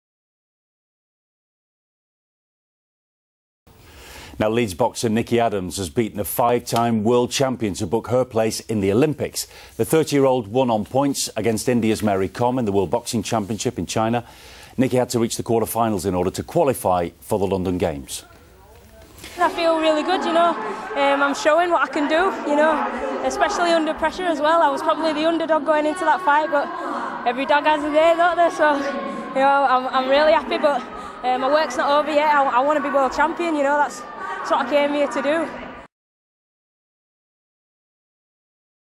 Nicola Adams Qualifies For The Olympics
Interview with Nicola Adams.